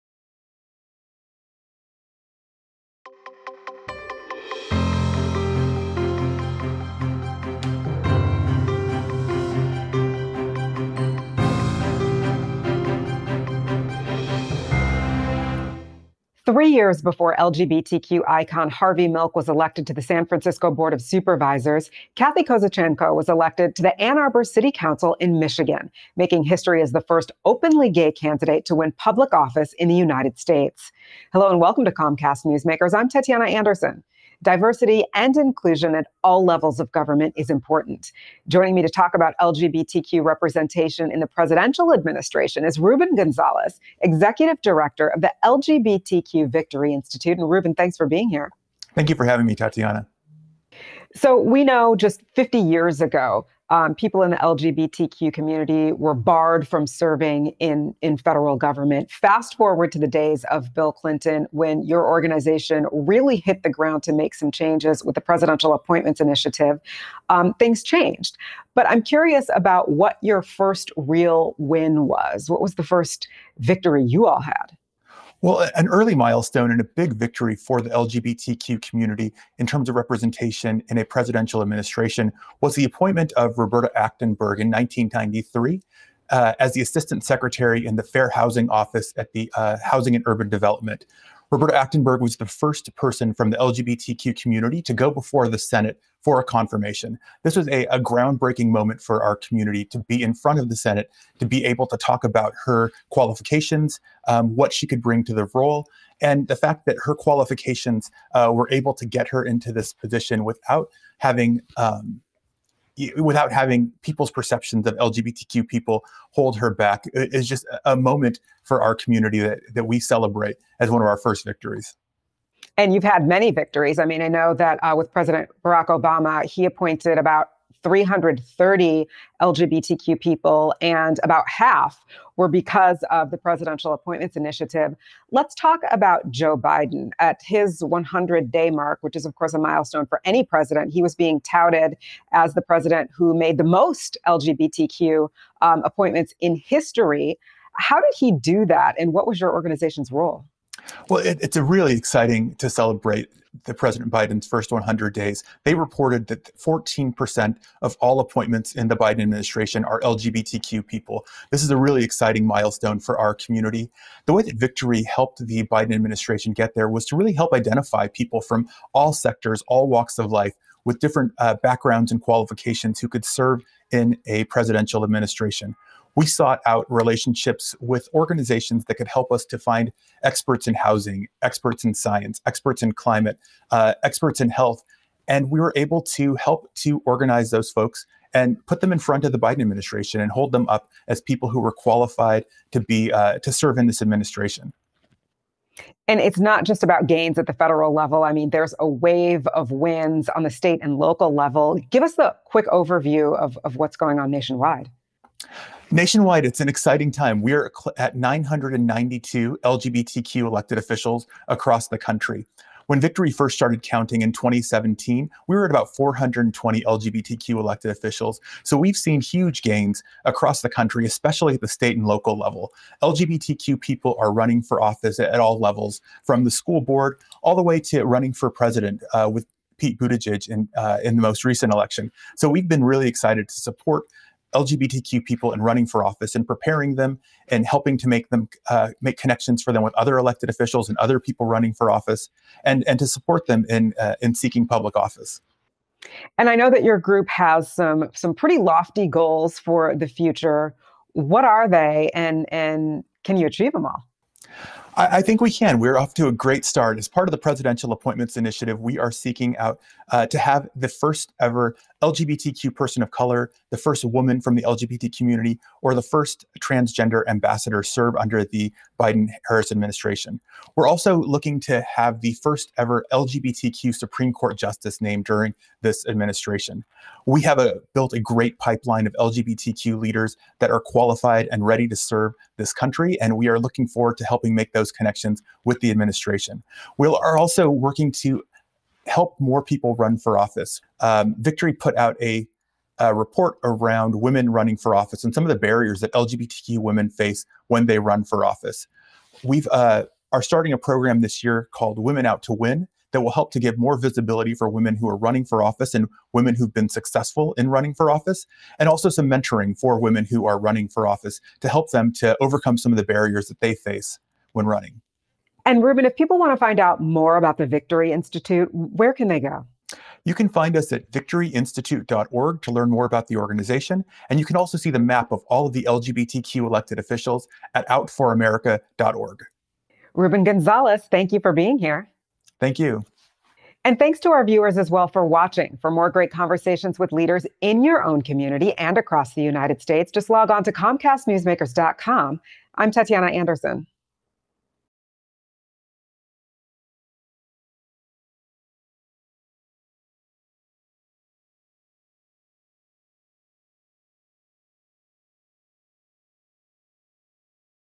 Gains in LGBTQ Political Representation
2020 saw more openly LGBTQ candidates than ever before run for public office, resulting in the highest level of LGBTQ representation in government, to date. At the time of this interview, there were more than 200 known LGBTQ appointees in the Biden administration.

Ruben Gonzales, Executive Director of the LGBTQ Victory Institute, joins Tetiana Anderson to discuss the gains made in LGBTQ representation.